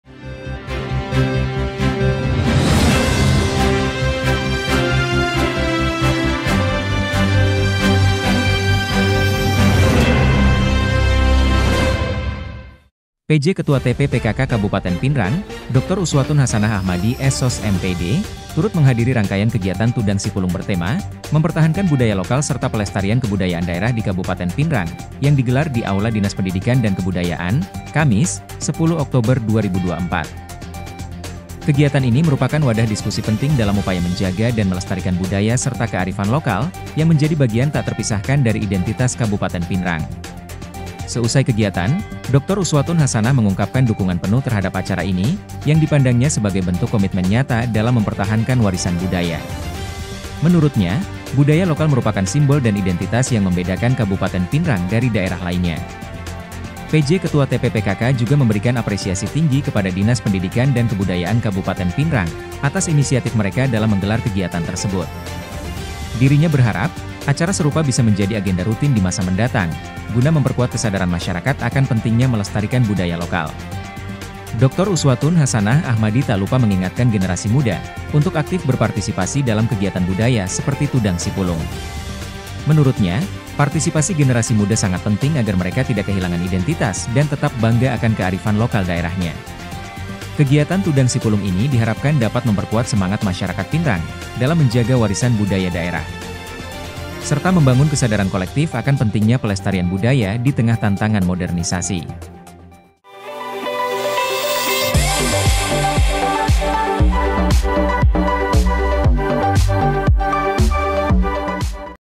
[PIC] Pj. Ketua TP PKK Pinrang Dukung Pelestarian Budaya Lokal dalam Tudang Sipulung
Pinrangkab.go.id — Pj. Ketua TP PKK Kabupaten Pinrang, DR. Uswatun Hasanah Ahmadi, S.Sos., M.Pd., turut menghadiri rangkaian kegiatan Tudang Sipulung bertema “Mempertahankan Budaya Lokal serta Pelestarian Kebudayaan Daerah di Kabupaten Pinrang” yang digelar di Aula Dinas Pendidikan dan Kebudayaan, Kamis (10/10).

Kegiatan ini merupakan wadah diskusi penting dalam upaya menjaga dan melestarikan budaya serta kearifan lokal yang menjadi bagian tak terpisahkan dari identitas Kabupaten Pinrang.

Seusai kegiatan, DR. Uswatun Hasanah mengungkapkan dukungan penuh terhadap acara ini, yang dipandangnya sebagai bentuk komitmen nyata dalam mempertahankan warisan budaya.

Menurutnya, budaya lokal merupakan simbol dan identitas yang membedakan Kabupaten Pinrang dari daerah lainnya.

“Saya sangat mengapresiasi kegiatan Tudang Sipulung ini. Ini bukan hanya tentang membicarakan budaya, tetapi juga tentang menjaga dan melestarikan warisan yang menjadi jati diri Kabupaten Pinrang. Budaya lokal adalah identitas kita, dan upaya seperti ini harus terus didorong agar tidak hilang di tengah arus globalisasi,” ungkapnya.

Pj. Ketua TP PKK juga memberikan apresiasi tinggi kepada Dinas Pendidikan dan Kebudayaan Kabupaten Pinrang atas inisiatif mereka dalam menggelar kegiatan tersebut.

Dirinya berharap acara serupa bisa menjadi agenda rutin di masa mendatang, guna memperkuat kesadaran masyarakat akan pentingnya melestarikan budaya lokal.

“Terima kasih kepada Dinas Pendidikan dan Kebudayaan yang telah menginisiasi acara ini. Saya berharap ke depan kegiatan seperti ini dapat terus berlanjut, sehingga kita bisa mewariskan nilai-nilai budaya kepada generasi berikutnya,” tambahnya.

DR. Uswatun Hasanah Ahmadi tak lupa mengingatkan generasi muda untuk aktif berpartisipasi dalam kegiatan budaya seperti Tudang Sipulung. Menurutnya, partisipasi generasi muda sangat penting agar mereka tidak kehilangan identitas dan tetap bangga akan kearifan lokal daerahnya.

“Generasi muda adalah pewaris budaya kita. Saya sangat berharap mereka ikut aktif dalam acara seperti ini, agar lebih mengenal budaya dan kearifan lokal kita. Jangan sampai kita kehilangan identitas sebagai generasi Pinrang yang kaya akan budaya,” tutupnya.

Kegiatan Tudang Sipulung ini diharapkan dapat memperkuat semangat masyarakat Pinrang dalam menjaga warisan budaya daerah, serta membangun kesadaran kolektif akan pentingnya pelestarian budaya di tengah tantangan modernisasi.(*/)

Foto & Release Collected By : Mahasiswa PPL IAIN Parepare (Amin,Fika & Rani)